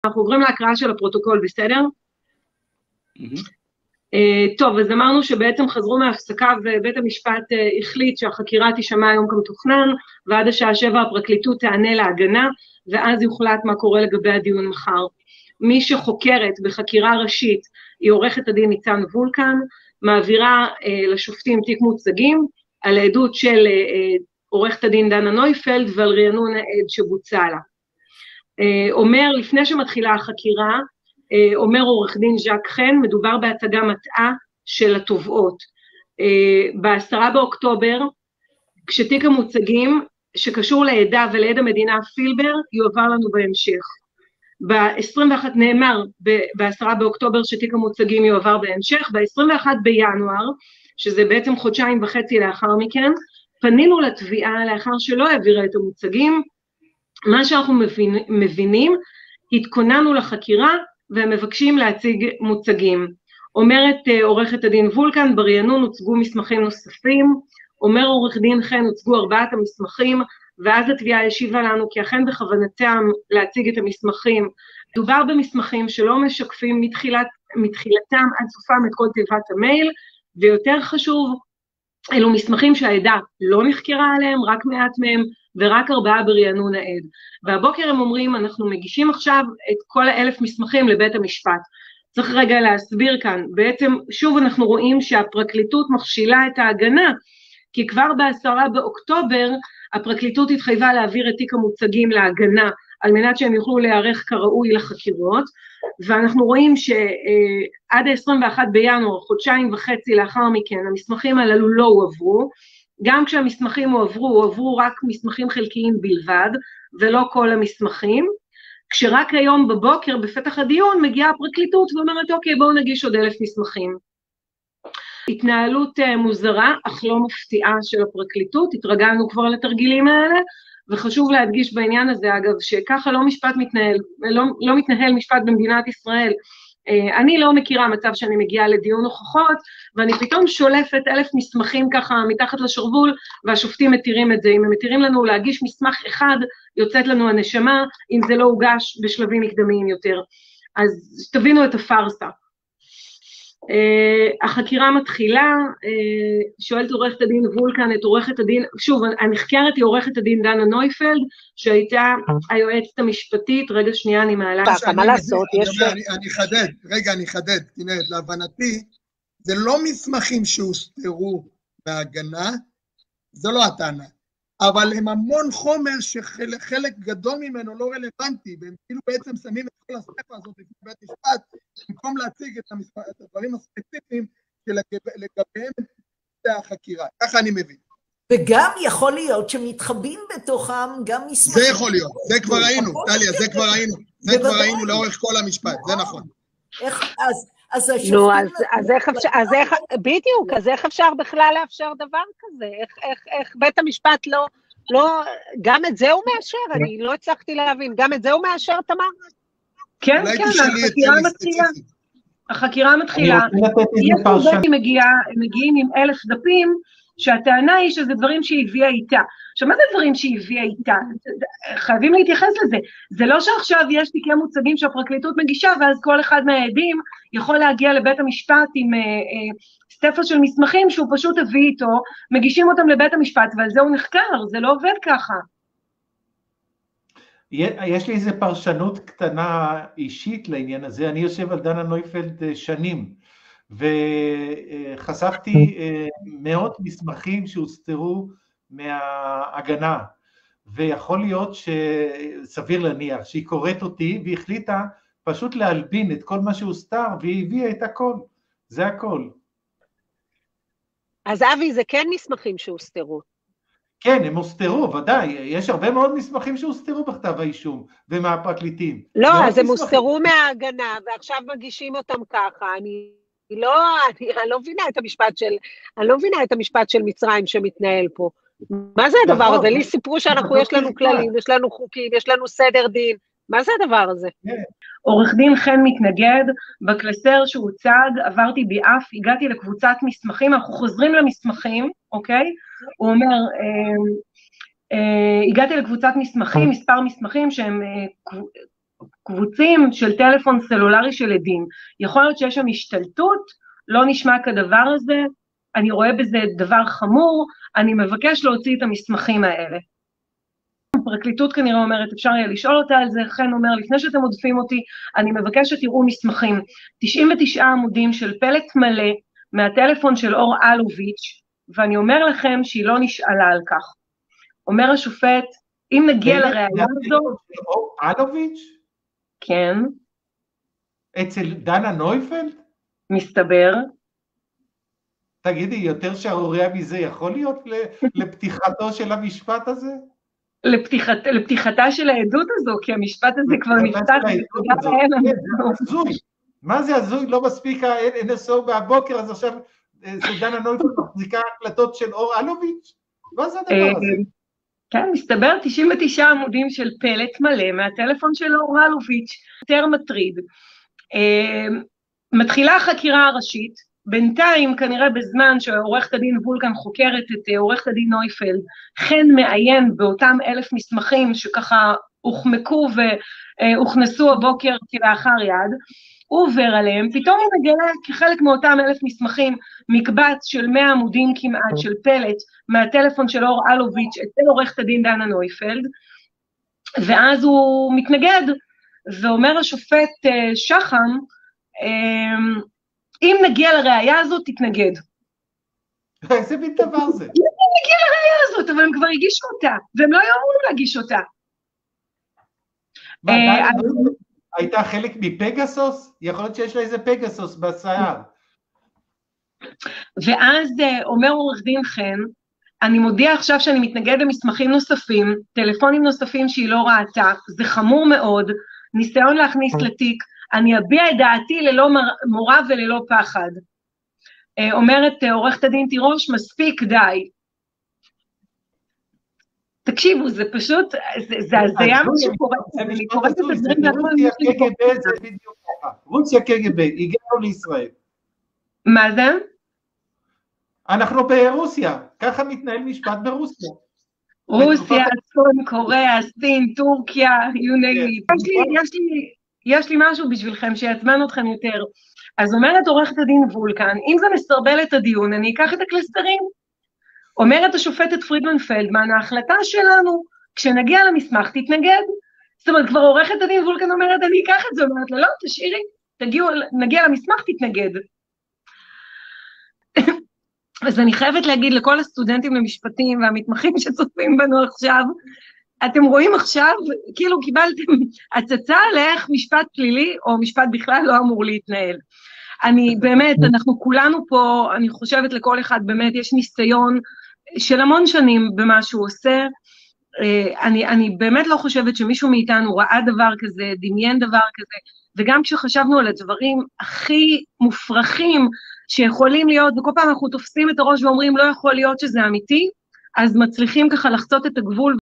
תרגיל של נויפלד, פירוט הטלפון של אור אלוביץ' והשופטים שמכשירים: זו דוגמא לאיך שלא צריך להתנהל משפט
תקציר חלק מיום העדות של עו"ד דנה נויפלד שהייתה היועמ"שית של משרד התקשורת בתקופת נתניהו .

עו"ד תמר ארבל מדווחת מבית המשפט:
הגיע עדה והביאה איתה 1000 מסמכים שלא הוגשו קודם לכן להגנה.
השופטים לא מתנגדים לכך!!

בראשי: אני לא מבינה מה זה המשפט המצרי שמתנהל כאן?!

עו"ד ארבל:
פרקליט ההגנה מגלה בדפים שהביאה נויפלל 99 עמודים המכילים פרוט הטלפון של אור אלוביץ'

ווייס:
איך בדיוק מגיעים דפים כאלה לדנה נויפלד שלא הייתה קשורה בחקירה?!
אבי ווייס: